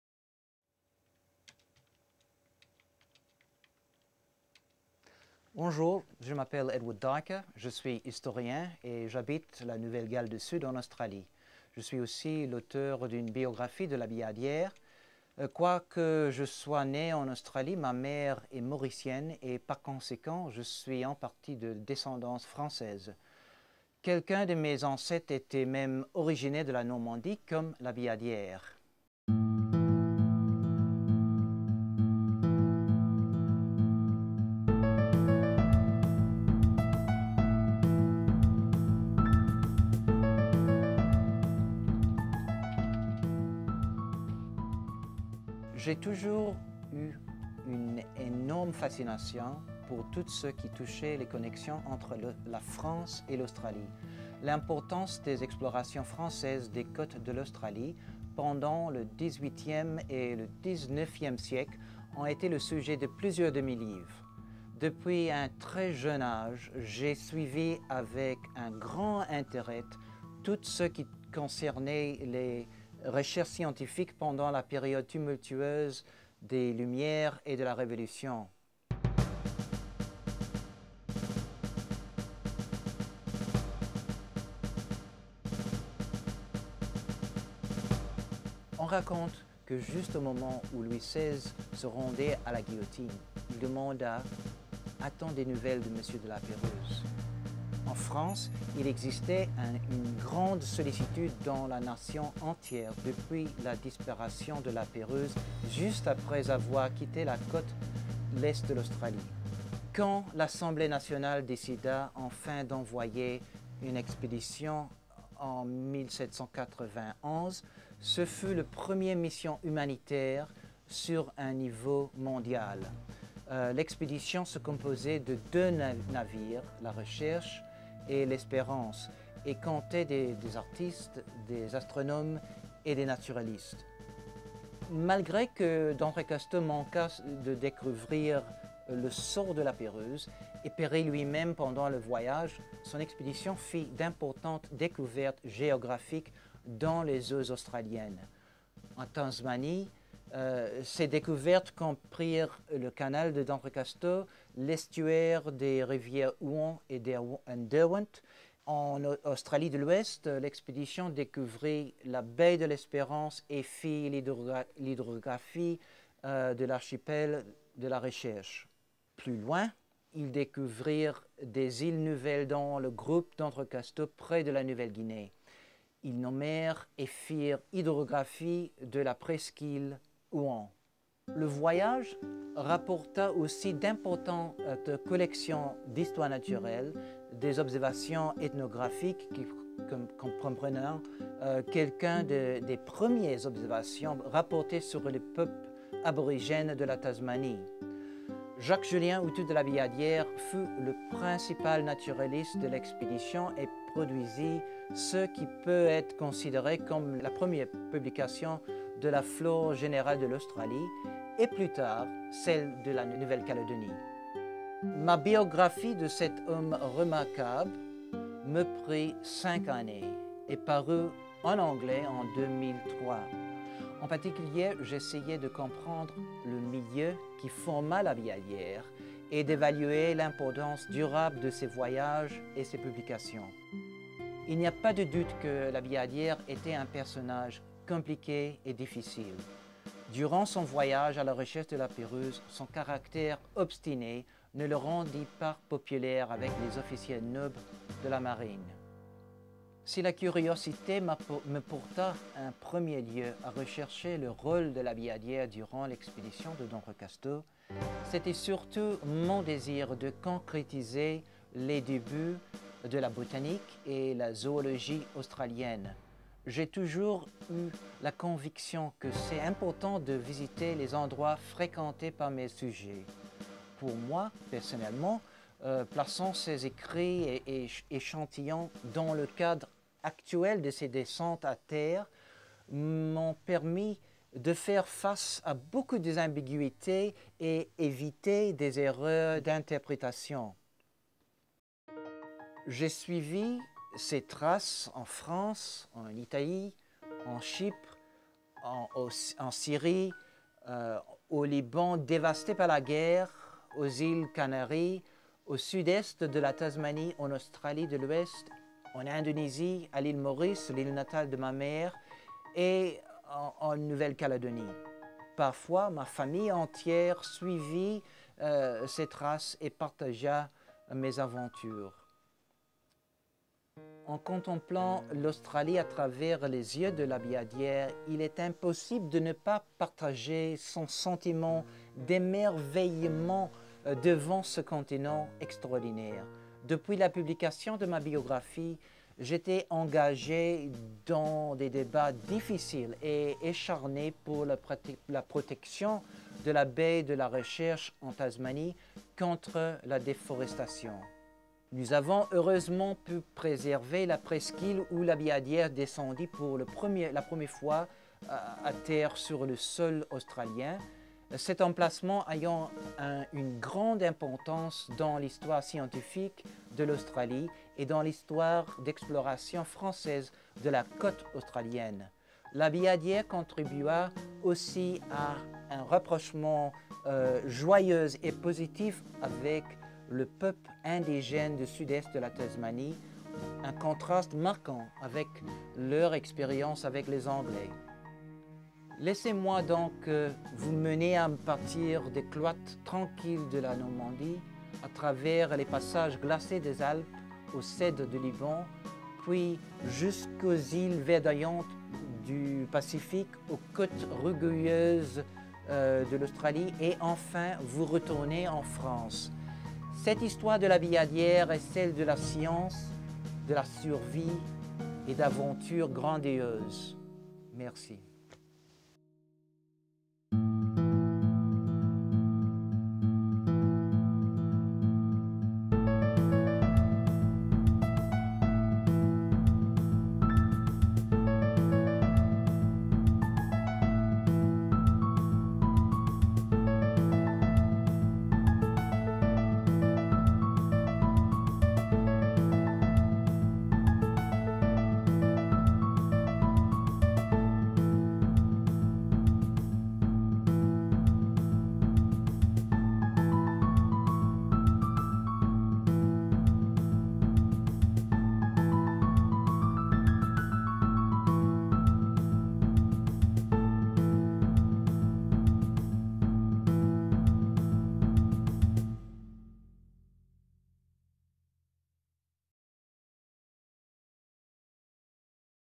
On the Trail of Labillardière
Promotional video for Professor Edward Duyker's book 'Citizen Labillardière' published by The Miegunyah Press in 2004.

Jacques-Julien Houtou de Labillardière was one of the great traveller-naturalists of the eighteenth century. He is most famous for his account of his voyage to the South Seas with Bruny d'Entrecasteaux in search of La Pérouse in 1791-93.

Labillardière's Relation was an international bestseller in its day, helping to usher the southern continent into the European imagination. During his visit to the south-western coast of New Holland and his two sojourns in Van Diemen's Land (Tasmania), Labillardière also laid the foundations for his magnificent Novae Hollandiae plantarum specimen (1804-06), which is justly recognised as the 'first general flora of Australia'. He was also the author of the first published flora of New Caledonia.

In researching this exciting and elegantly written biography, Edward Duyker revisited many of the naturalist's landfalls around the world. He also examined a wide range of archival and museum collections to piece together Labillardière's correspondence and observations. The result is the first comprehensive study of the naturalist, revealing a committed republican who was shaped by the turbulent years of revolutionary and Napoleonic France.

Dr Duyker ranges widely: from the tranquil cloisters of Normandy to the pillaged libraries and museums of Italy, from the Cedars of Lebanon to the verdant islands of the Pacific, from the frozen passes of the Alps to the parched shores of New Holland. This is a story of science, survival and a grand adventure.